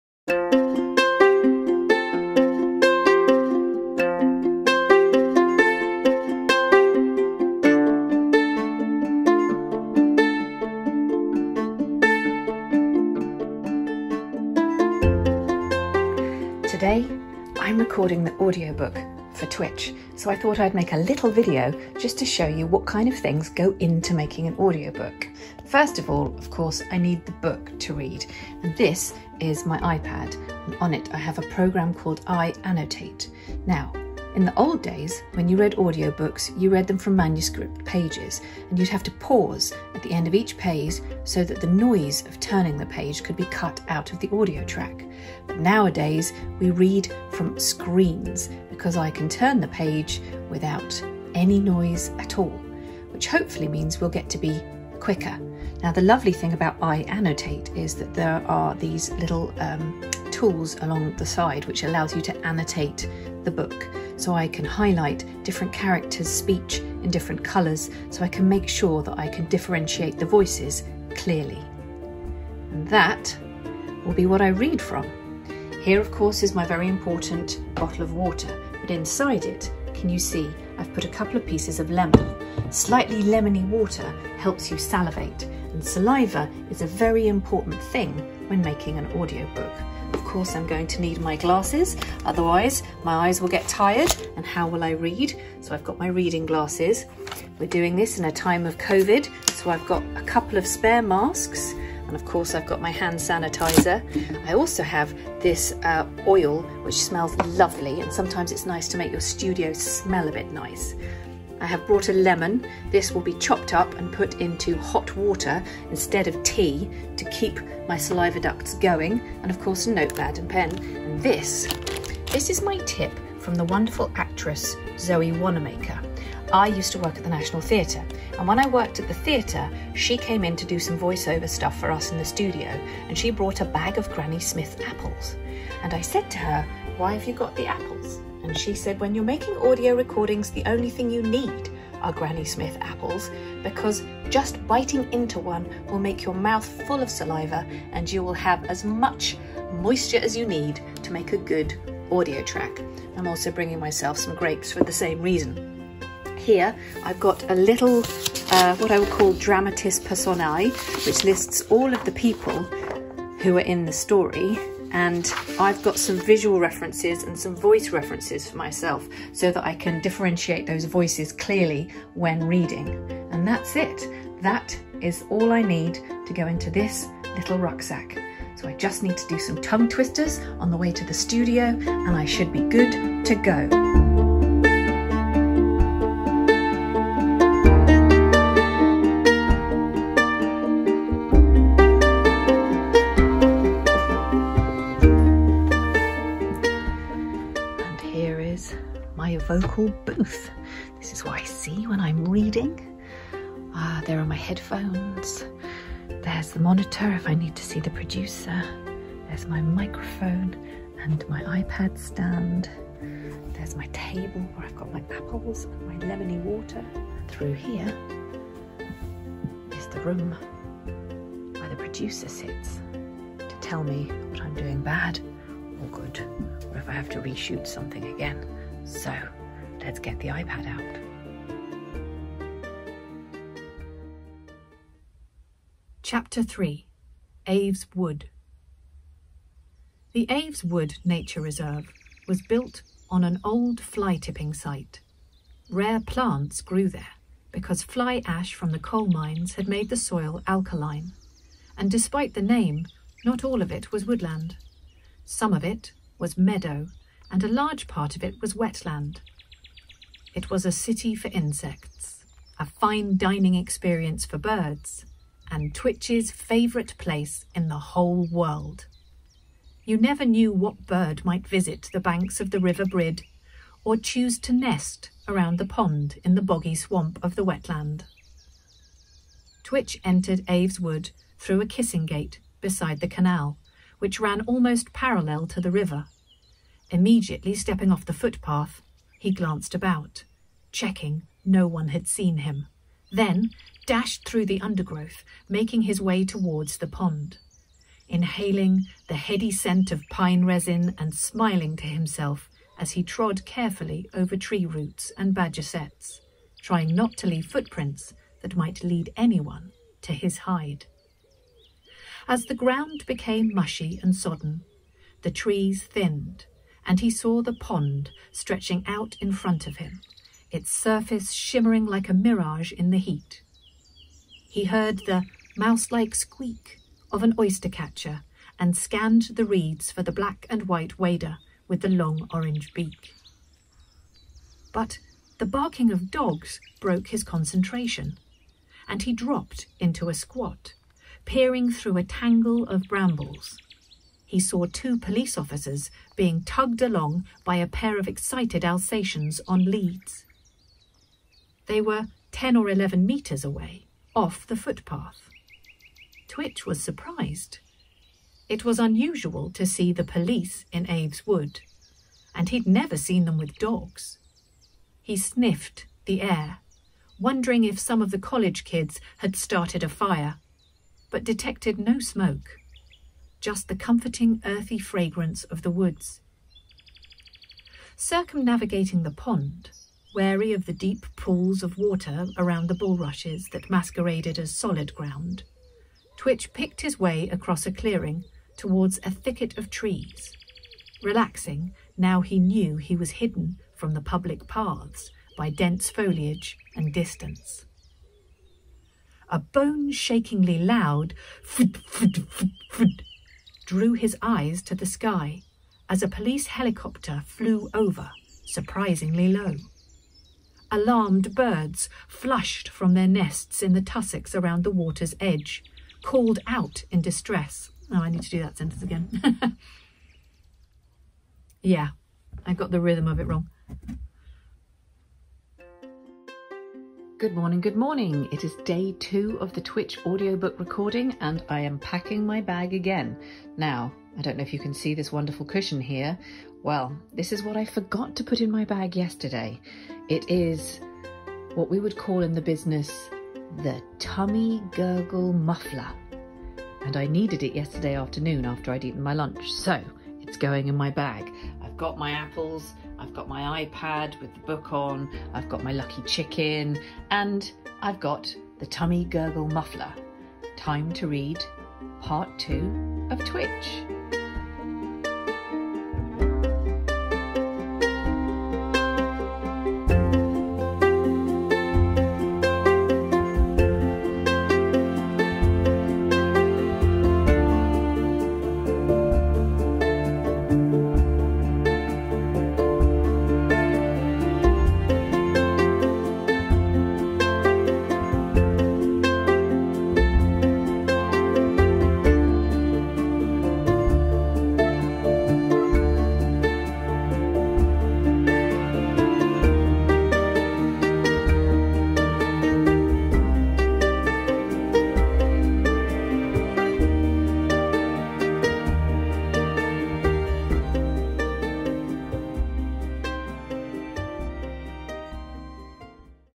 Making of the Audiobook of TWITCH (Author M. G. Leonard takes us behind the scenes)
Have you ever wondered how audiobooks are made?

Author M. G. Leonard takes us behind the scenes as she records the audiobook for her middle-grade adventure TWITCH - a story of a birdwatcher who sets out to outwit a convict.

You can find M. G. Leonard on all social media sites and get all the news about her newest books and appearances from her